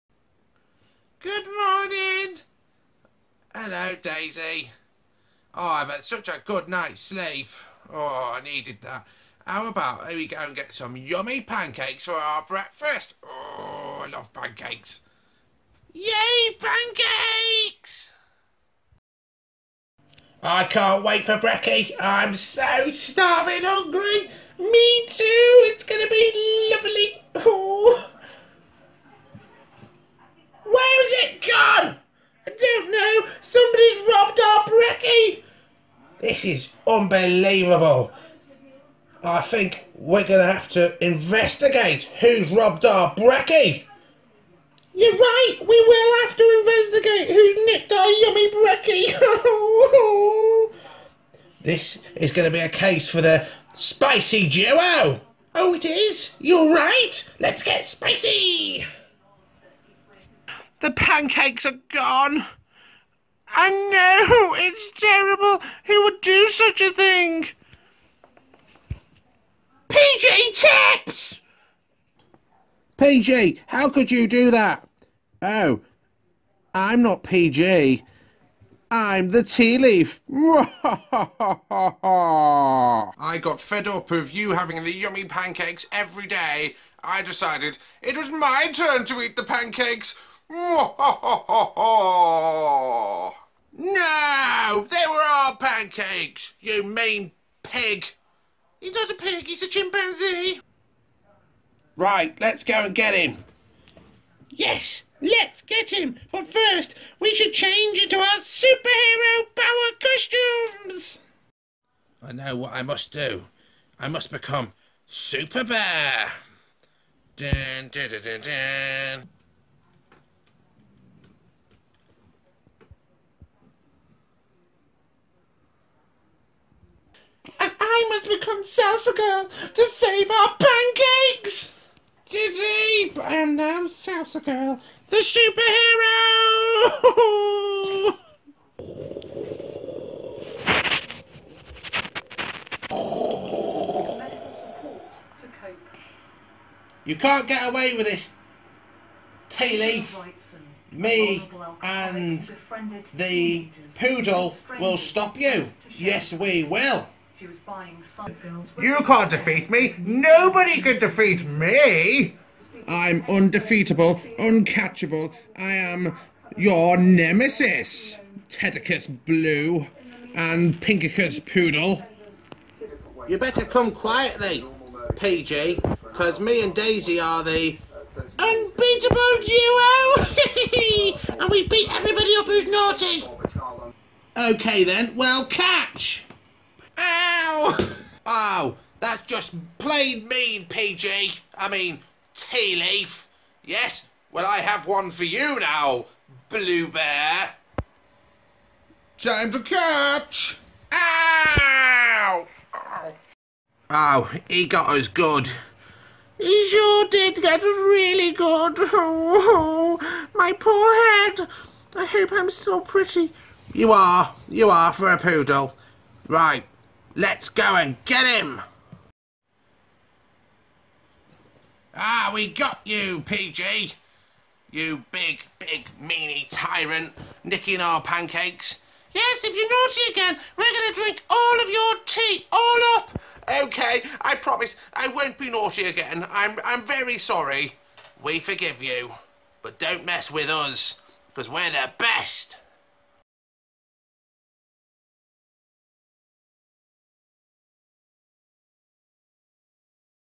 Blue Ted & Daisy: Hereos Begin
The Unbeatable Duo face their most dangerous nemesis the Tea Leaf, in a hilarious short film about pancake vengeance!